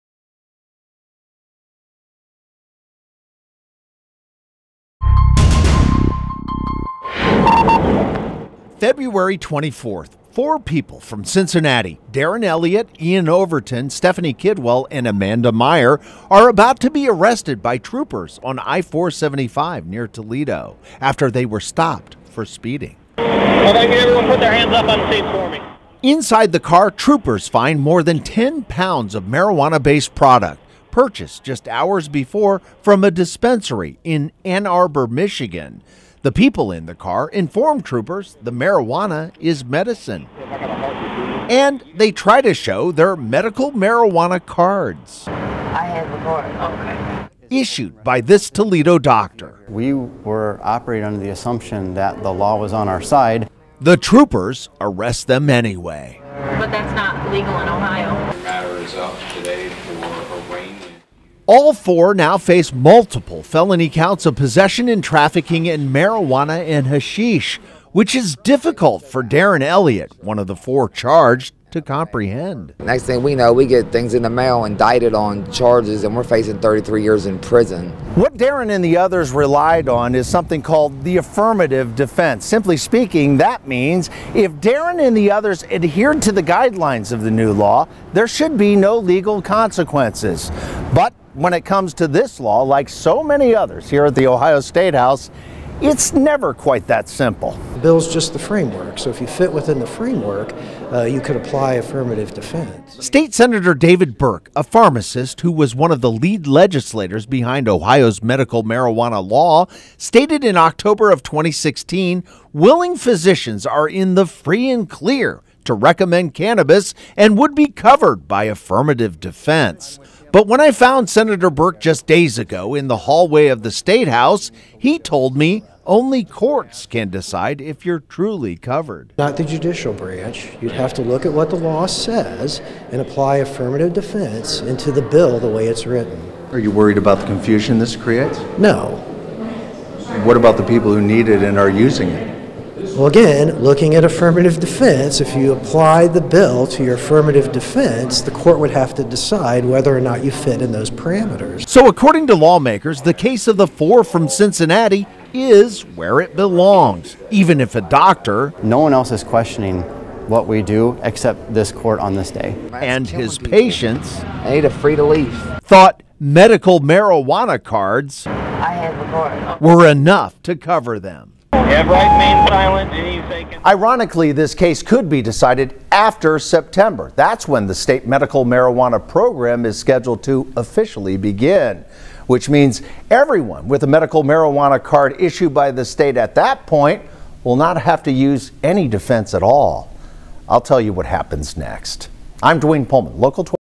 OHIO SPJ CRIMINAL JUSTICE "Medicinal or Criminal"
Dozens of other media outlets tried to get them, but only WKRC-TV’s Duane Pohlman aired reports involving exclusive interviews with a group of four people from Cincinnati who were arrested along an interstate and charged with felony drug counts after having and using what they claimed was medical marijuana.
Pohlman and photographer Eric Frisbee traveled 4 hours to Toledo to cover their court hearing and more than two hours to Ohio Statehouse to interview the lawmaker who helped author Ohio’s medical marijuana law. Our coverage highlights the very real concern and confusion over drug laws that have changed, while the enforcement has not.